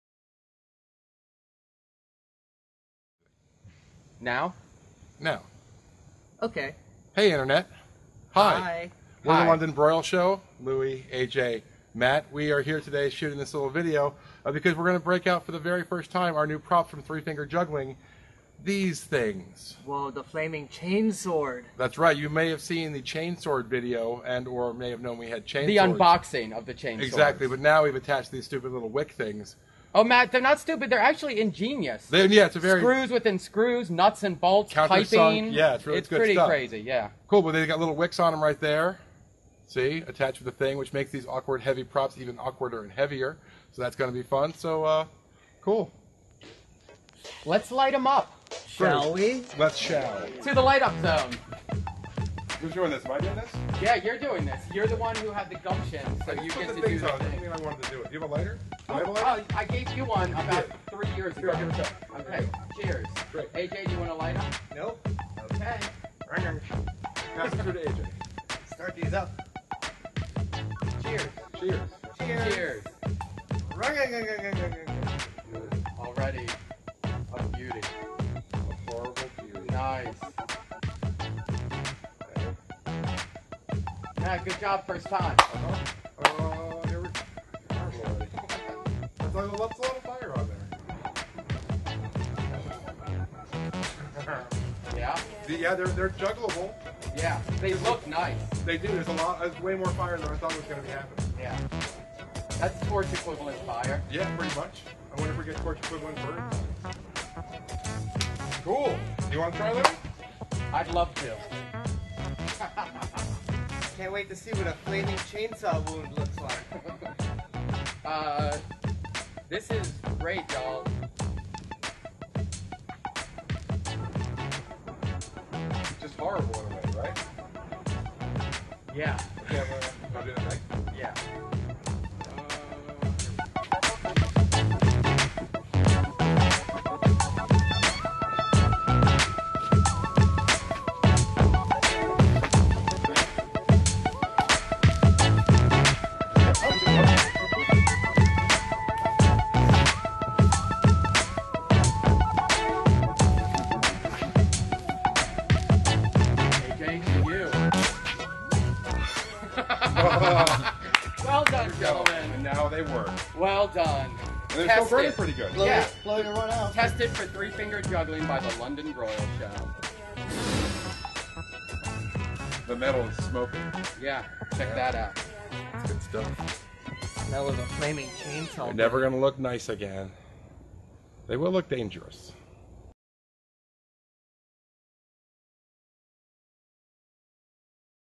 The London Broil Show - Flaming ChainSwords Juggling
The London Broil Boys lighting up the ChainSwords for the first time.
The London Broil Show - a perfect blend of juggling and hanging out with friends. See the show at a Renaissance Festival near you!

Filmed at the North Carolina Renaissance Festival 2016.
Music from the YouTube Music Library - Otis Mcdonald.